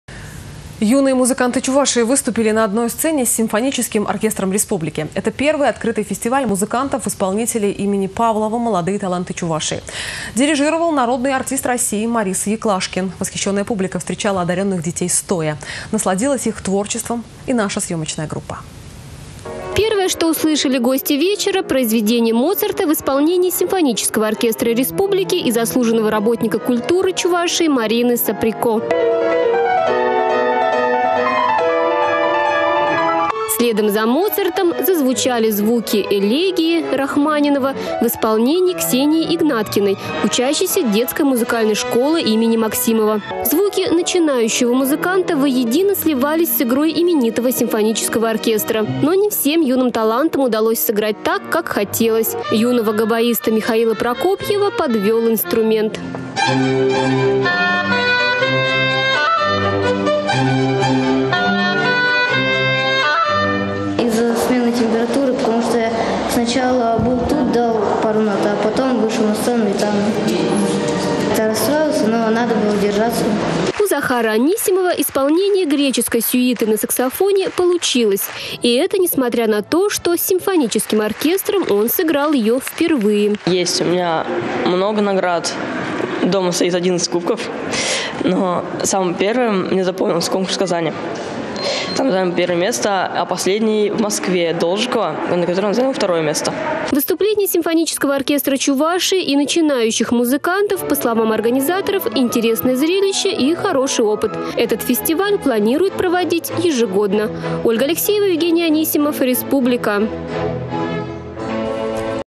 Первый открытый фестиваль музыкантов-исполнителей «Молодые таланты Чувашии»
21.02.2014
Юные музыканты Чувашии выступили на одной сцене с симфоническим оркестром Республики. Это первый открытый фестиваль музыкантов-исполнителей имени Павлова «Молодые таланты Чувашии». Управлял выступлением народный артист России Морис Яклашкин. Восхищенная публика встречала одаренных детей стоя.